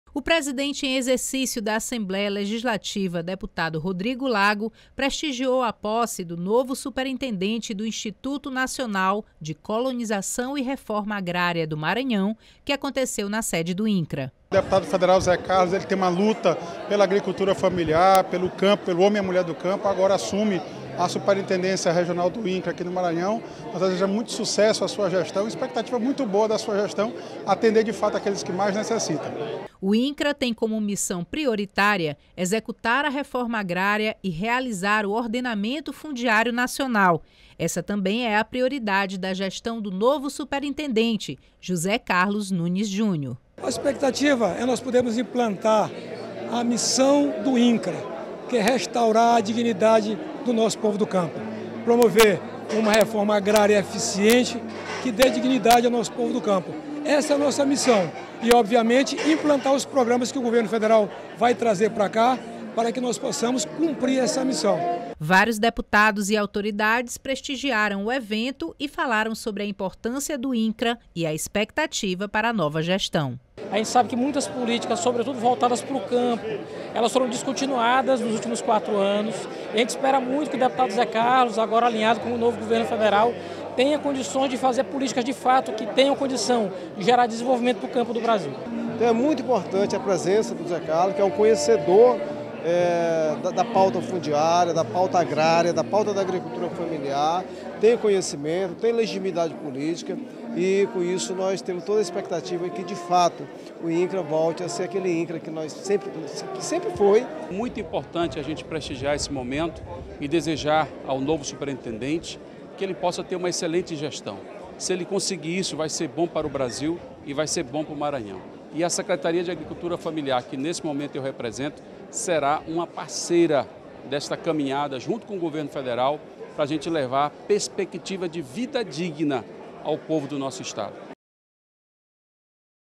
Assembleia prestigia posse do novo superintendente do Incra José Carlos
A solenidade aconteceu na sede da autarquia federal, em São Luís. O novo gestor é formado em Engenharia Civil e já foi deputado estadual e federal, quando atuou com foco na agricultura familiar